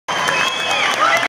Sergio Rodriguez 3.MOV
DESPUES DE LA VICTORIA, EL AGRADECIMIENTO A SUS MAESTROS DE BRAZILIAN JIU-JITSU ACADENY EN SANTA FE NM , LA MEJOR!! POR CIERTO!!